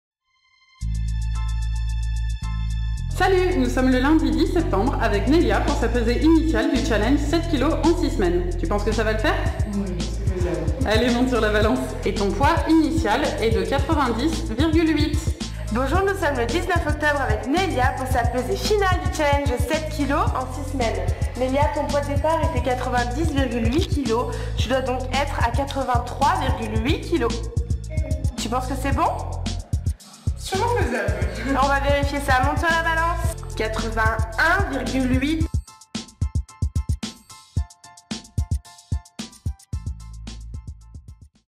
Transformation physique rapide Lyon
Nelia a perdu 9,000 kgs en 6 semaines à la Training Academy